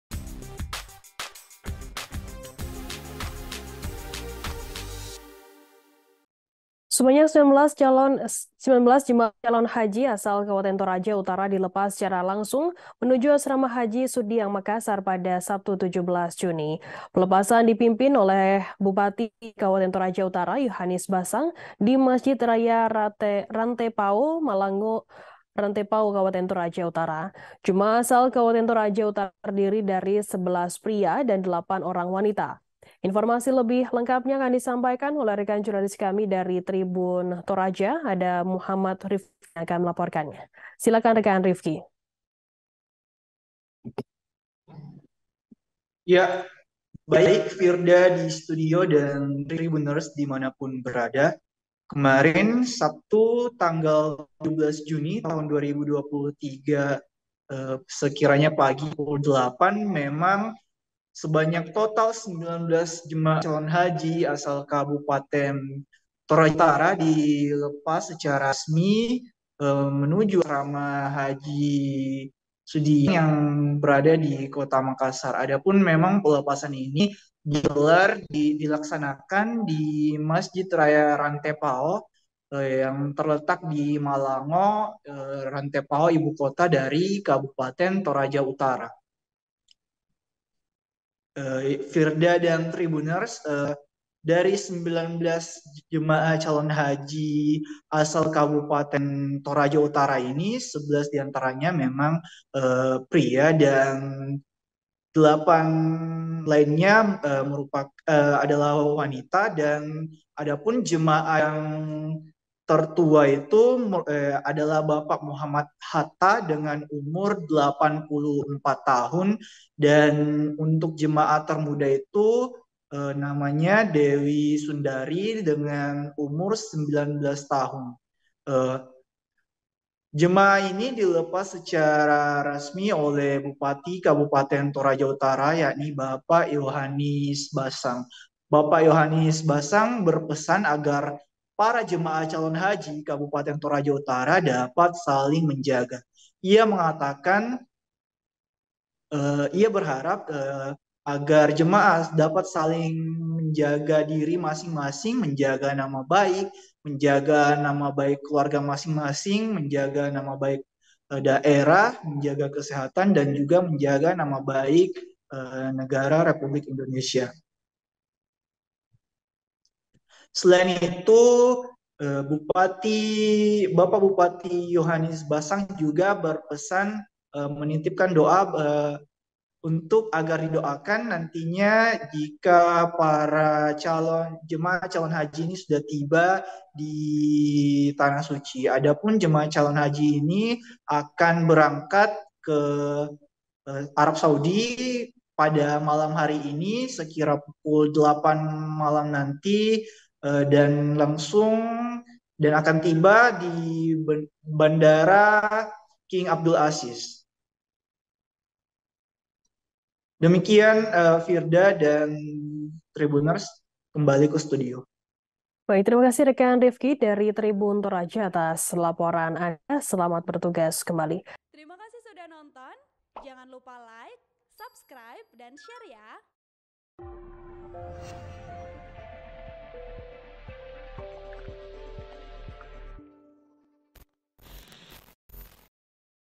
19 Jamaah Calon Haji Asal Toraja Utara Dilepas Oleh Ombass Menuju Asrama Haji Sudiang Makassar
TRIBUN-VIDEO.COM - Sebanyak 19 jemaah calon haji asal Kabupaten Toraja Utara dilepas secara langsung menuju Asrama Haji Sudiang, Makassar hari ini, Sabtu (17/6/2023).

Pelepasan dipimpin langsung Bupati Kabupaten Toraja Utara, Yohanis Bassang, di Masjid Raya Rantepao, Malango', Rantepao, Kabupaten Toraja Utara.

Jemaah asal Kabupaten Toraja Utara terdiri dari 11 pria dan delapan orang wanita.

Dalam sambutannya, Yohanis berpesan agar para jemaah calon haji Kabupaten Toraja Utara dapat saling menjaga. (*)